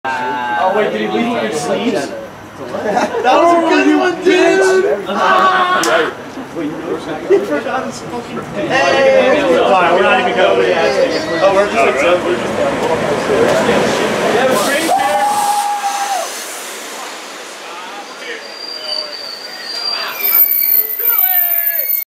09/12/14 SEPTA 578 arrives Radnor
My train to the Cabrini Night at the Phillies game arrives. Went up with some friends from campus.